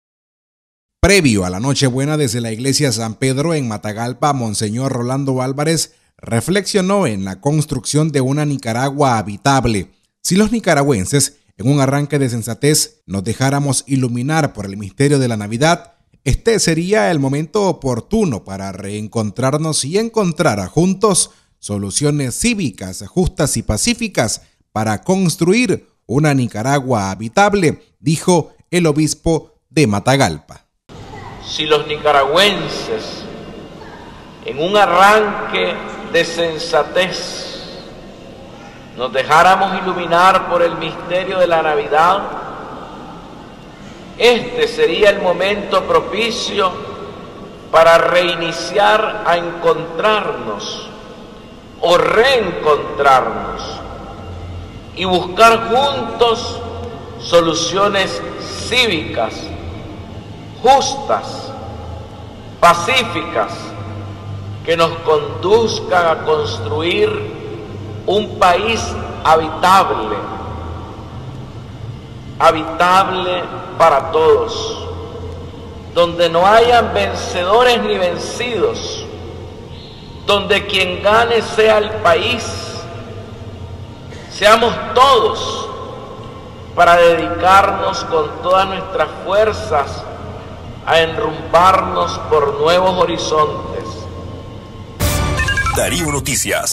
Mons. Álvarez: “Es el momento para construir una Nicaragua habitable”
Previo a la noche buena desde la iglesia de San Pedro en Matagalpa, Monseñor Rolando Álvarez reflexionó en la construcción una Nicaragua habitable. “Si los nicaragüenses en un arranque de sensatez nos dejáramos iluminar por el misterio de la Navidad, este sería el momento oportuno para reencontrarnos y encontrar juntos soluciones cívicas, justas y pacíficas para construir una Nicaragua habitable”, dijo el obispo de Matagalpa.